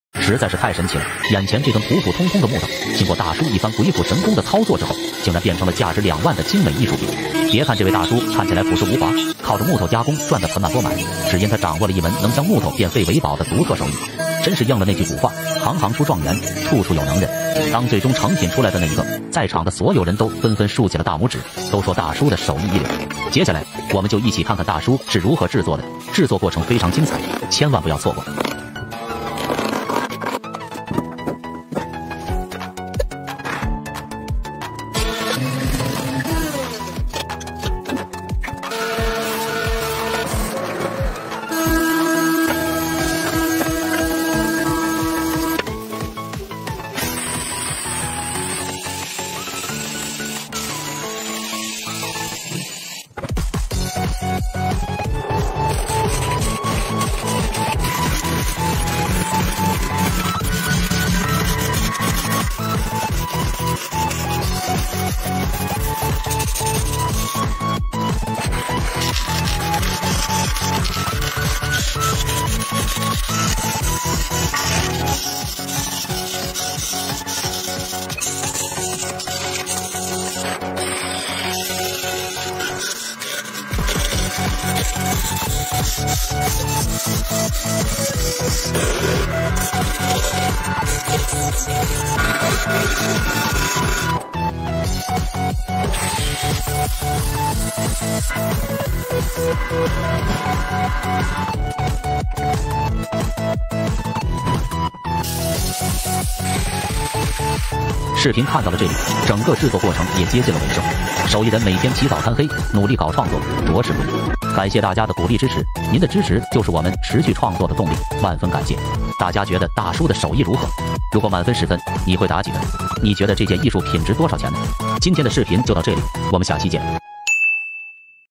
005 After being processed by the uncle, an ordinary piece of wood becomes extremely exquisite
005 After being processed by the uncle, an ordinary piece of wood becomes extremely exquisite